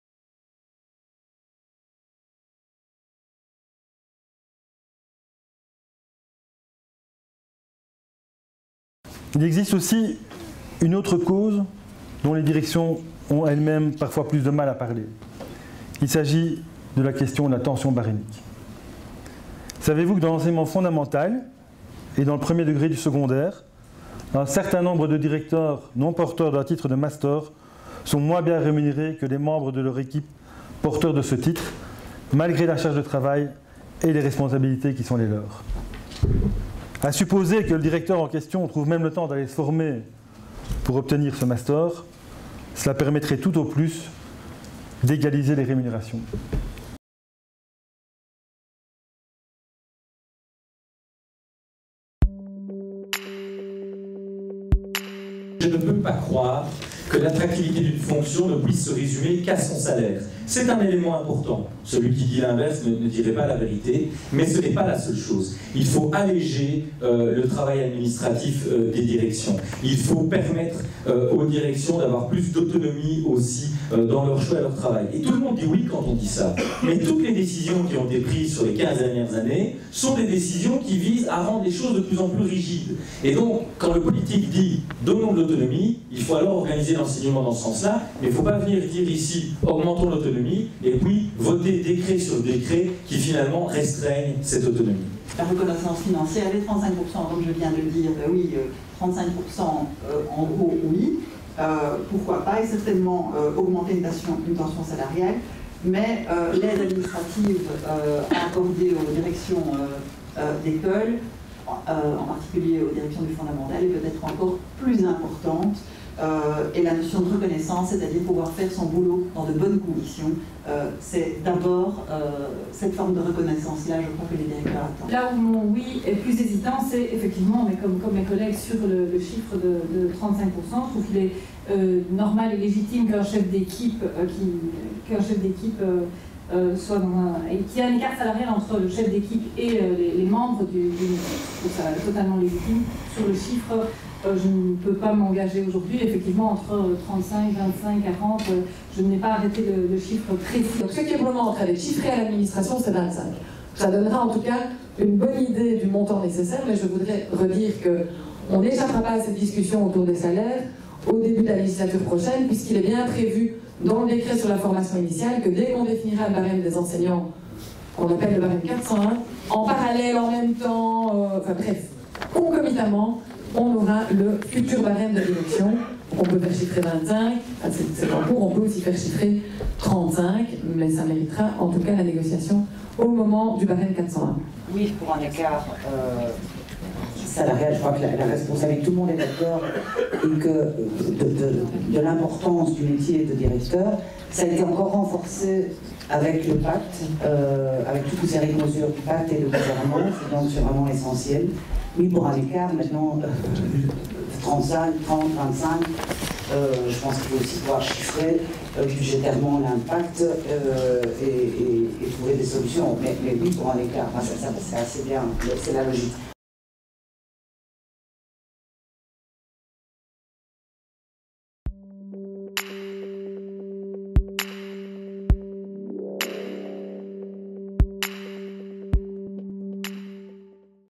Priorités 2019-2024 (4/7)
L’enseignement catholique formule 7 grandes priorités pour la prochaine législature. Priorité numéro 4 : « Assurer l’attractivité des fonctions de direction ». Dans cette séquence enregistrée lors de la journée d’étude du SeGEC en mars dernier à Namur, les représentants des 5 principaux partis francophones, se prononcent sur une question directement en lien avec la thématique.